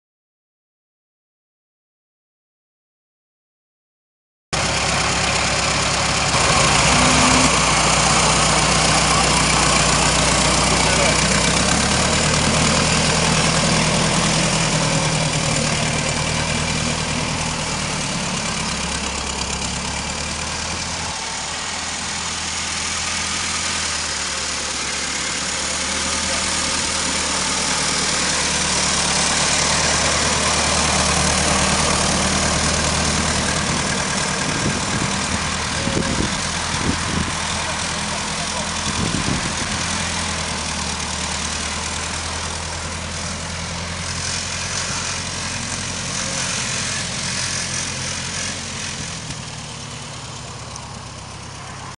Matane Cédric conduit son Ford model T préféré
Matane Ford model T touring 1926